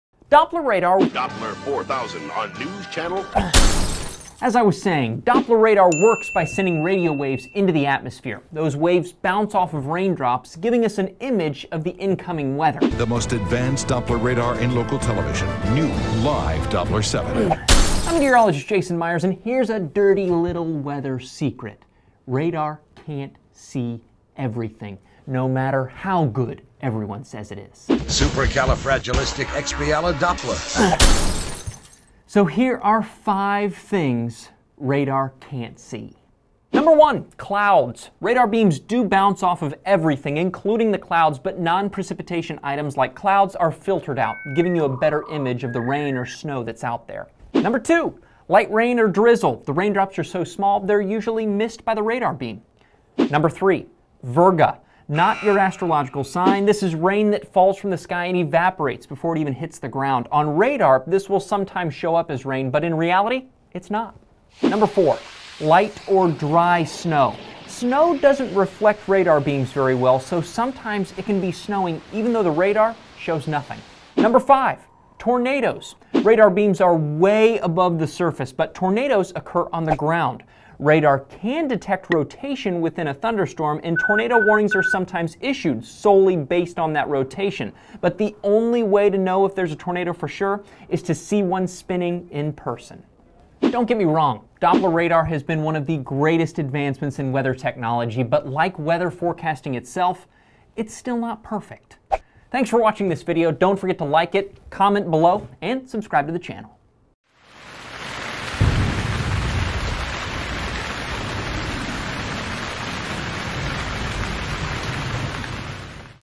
5 Things Radar can't see
The Doppler Radar is one of the greatest advancements in weather technology, but it's not perfect. There are some things it just can't see.